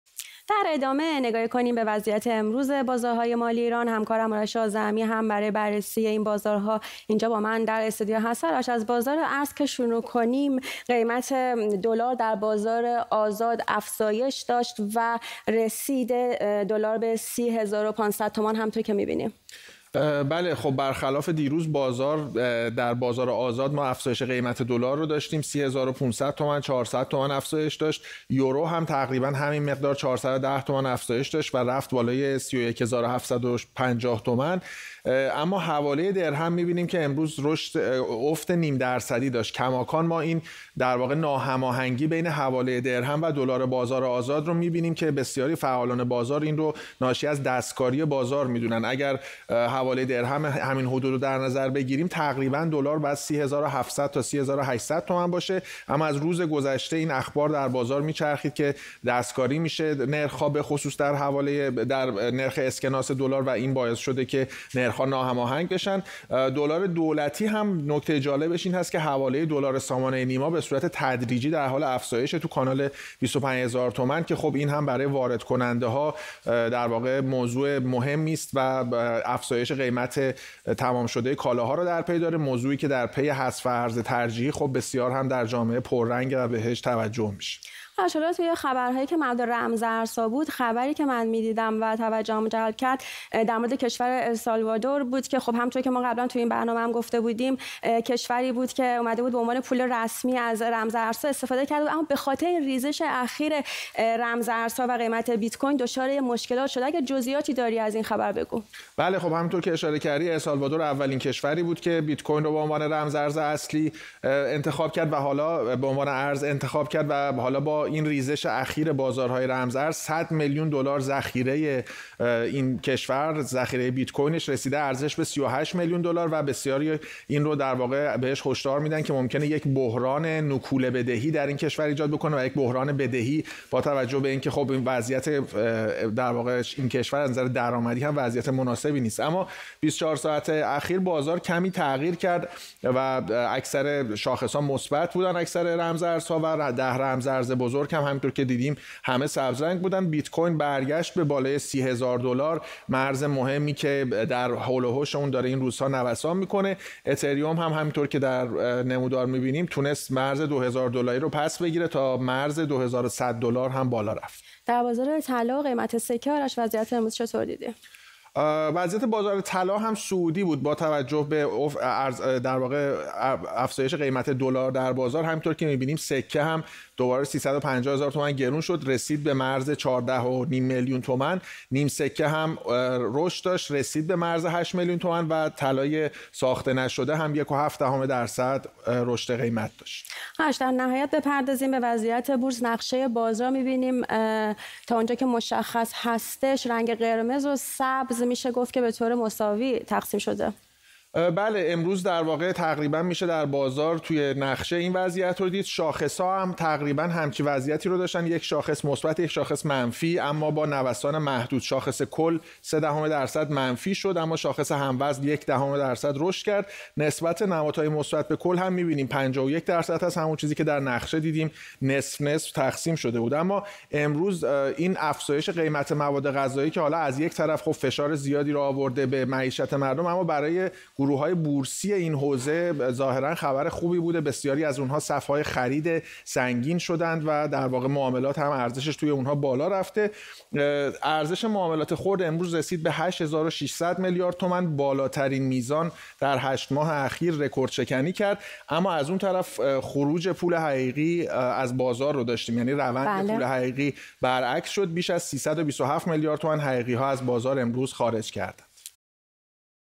دلار به ۳۰ هزار و ۵۰۰ تومان رسید، رمزارزها صعودی شدند
بررسی بازارها با آرش آزرمی، ایران اینترنشنال